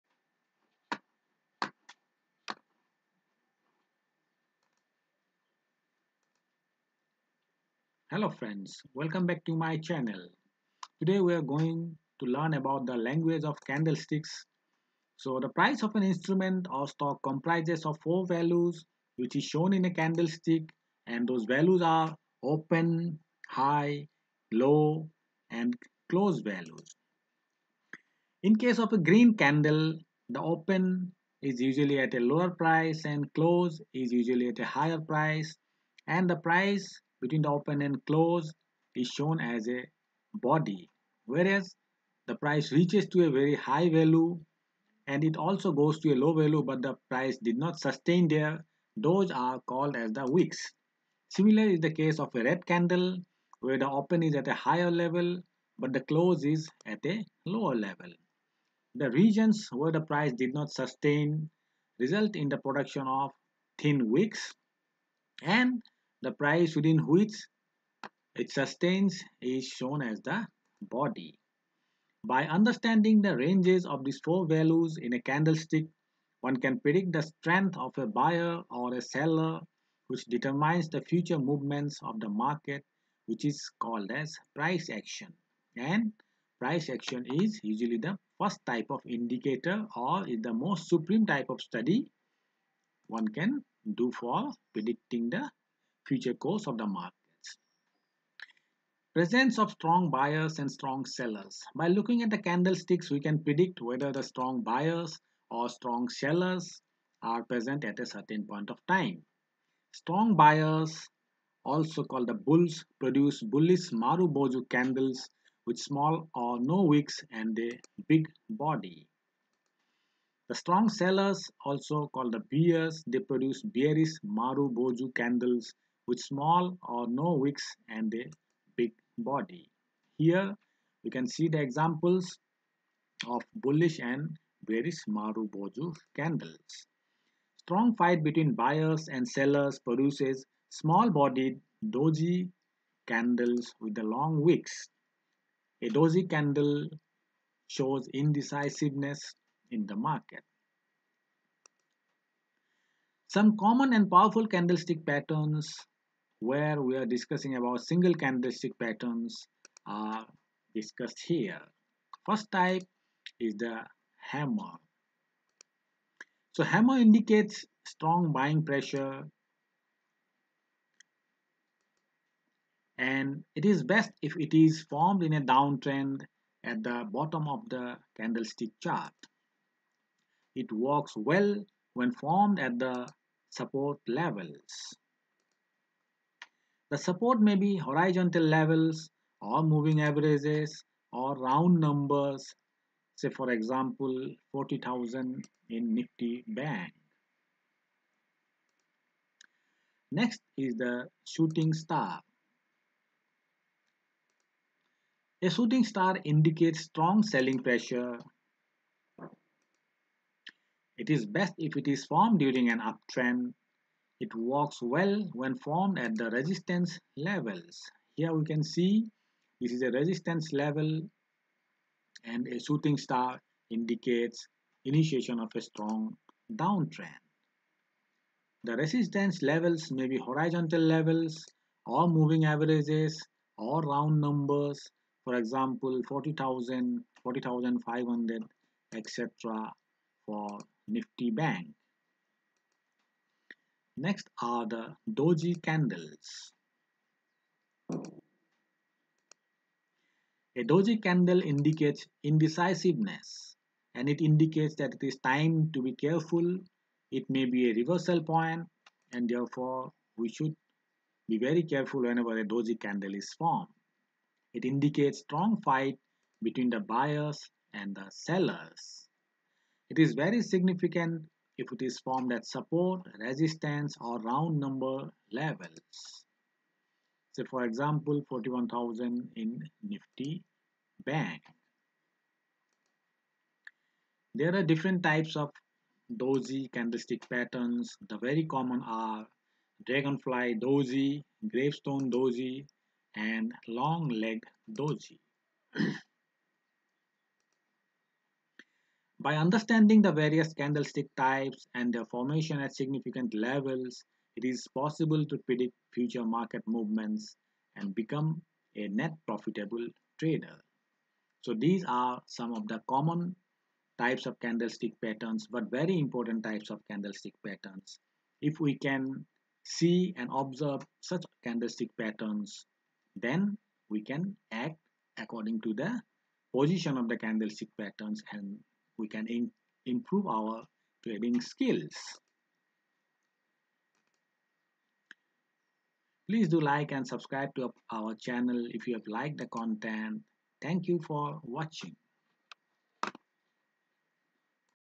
Language of Candlesticks Part I- Marubozu, Hammer, Shooting Star and Doji
The candlesticks are the ultimate indicators of price. Mastering candlestick patterns require identifying them and take positions according to their shape, size and location. Some important patterns are Marubozu, Hammer, Shooting Star and Doji Candlestick patterns.